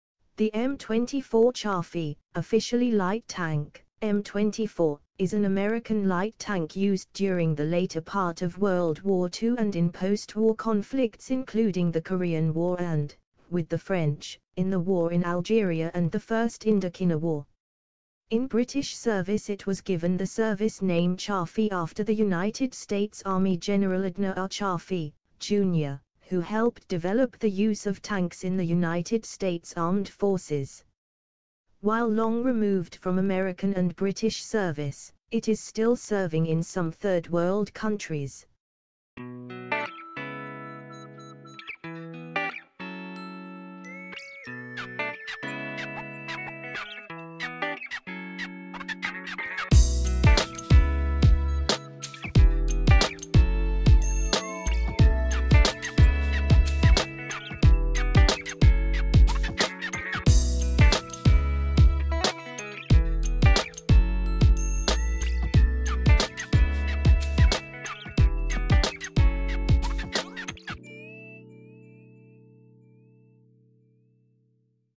EKW C-35 - Swiss biplane - HD
The EKW C-35 was a Swiss biplane that filled in as a surveillance and ground-assault airplane during the 1930s and 1940s. It was planned and worked by the Swiss Government Development Works (K+W) in Thun, Switzerland. The airplane had a Hispano-Suiza HS-77 motor, which provided it with a most extreme speed of 335 km/h and a scope of 750 km. It was furnished with a 20-mm gun, three assault rifles, and up to 100 kg of bombs. The EKW C-35 was utilized by the Swiss Aviation based armed forces until 1954, when it was supplanted by more present day airplane.

Thanks !